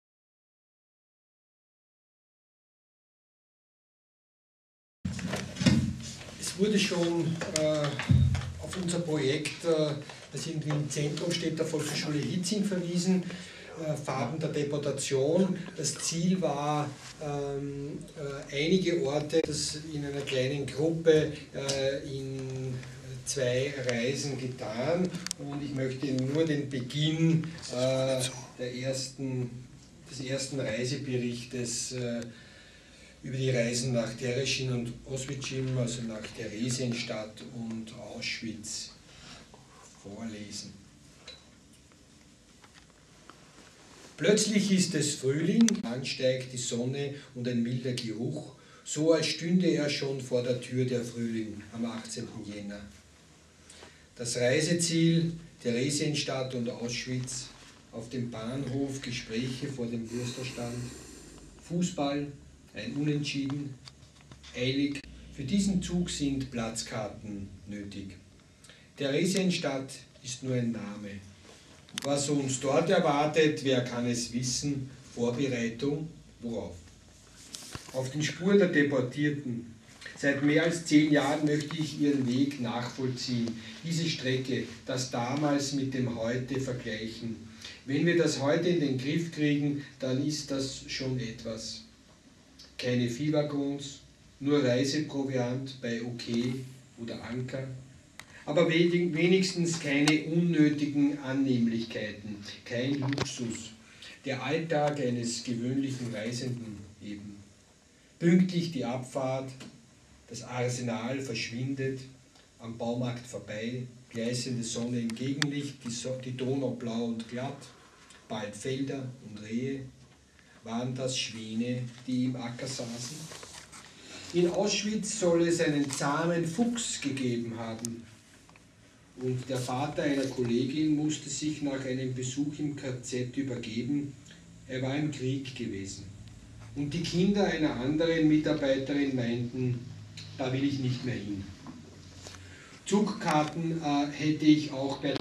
Robert Streibel: Auf der Suche nach den Juden aus Hietzing.
Ausschnitt aus der Lesung in der Österreichischen Gesellschaft für Literatur am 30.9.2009 im Rahmen der Präsentation der Spurensuche "Hinter den Mauern des Vergessenes".